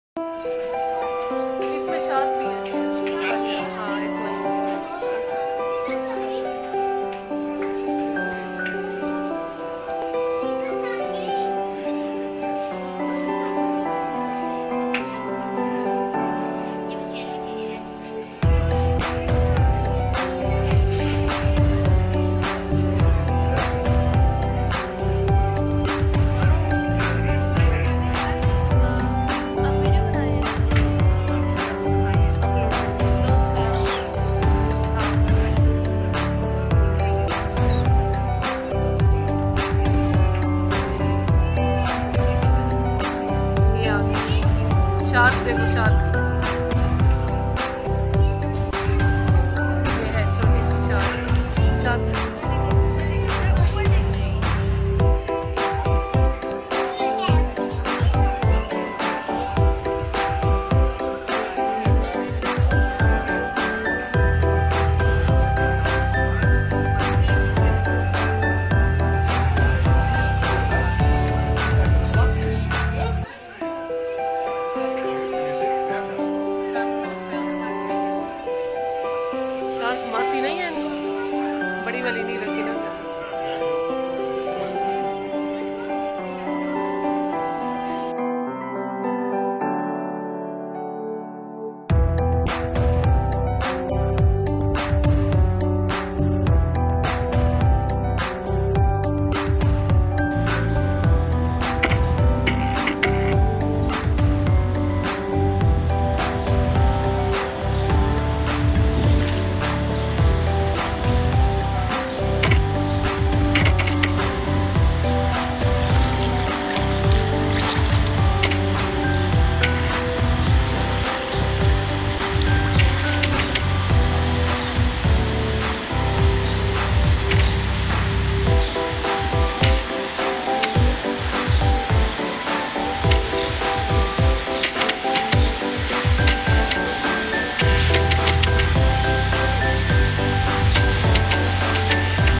300 Kilo leatherback turtle | shark | Zoomarine || Portugal
In their latest press release Zoomarine have announced that the 300 Kilo leatherback turtle rescued after being caught in fishing nets, has been named Quinas. The FB vote had more than 1750 participants!